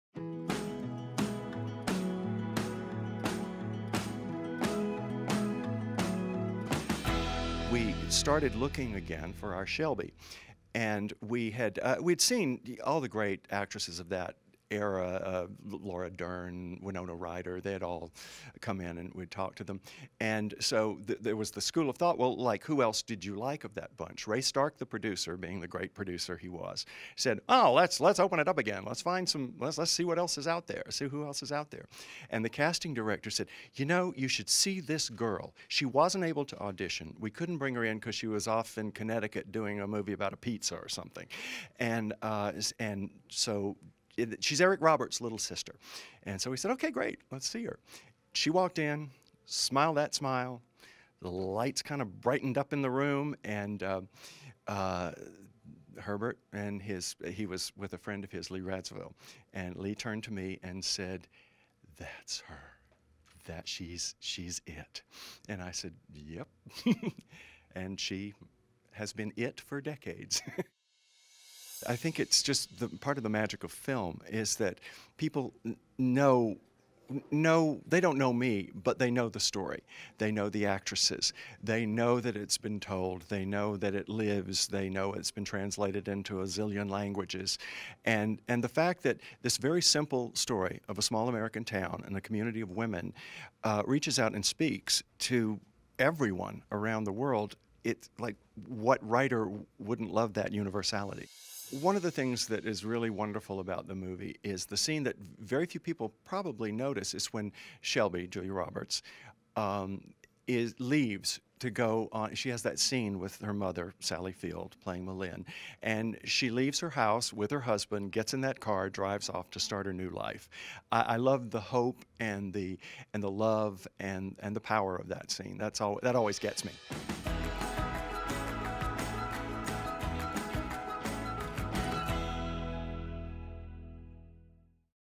STEEL MAGNOLIAS ('89) writer Robert Harling at the 2019 TCMFF
"Steel Magnolias" writer Robert Harling talks casting newcomer Julia Roberts in the classic film.